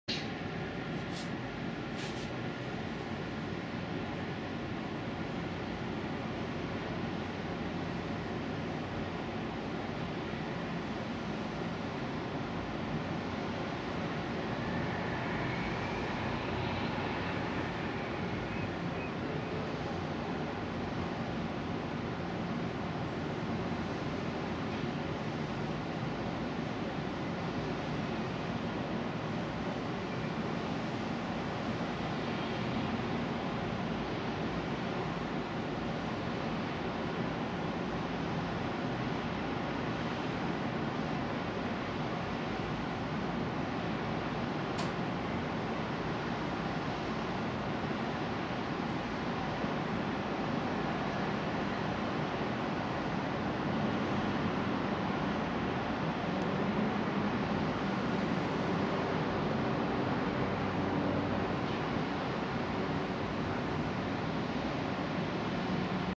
Floatplane Coming Vancouver, Canada
Floatplane, probably from Whistler, is coming to downtown Vancouver on April 11, 2013.